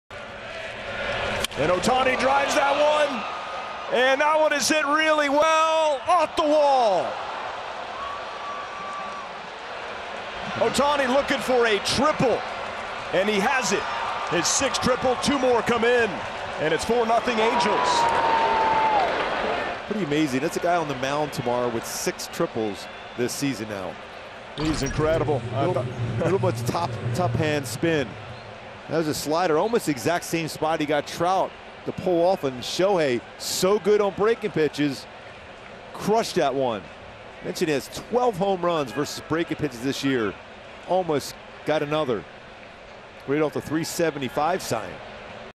Shohei Ohtani 2-run Triple: 8/26/2022
Shohei Ohtani slugs a two-run triple off the wall in right field to extend the Angels' lead to 4-0 in the top of the 2nd inning.

Pitcher: Mitch White (Blue Jays)